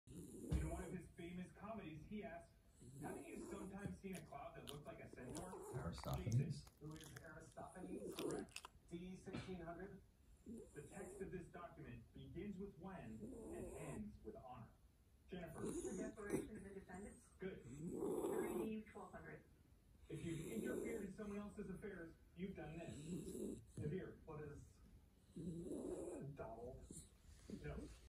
Sleepy Westie
Birdie just can’t help falling asleep with you.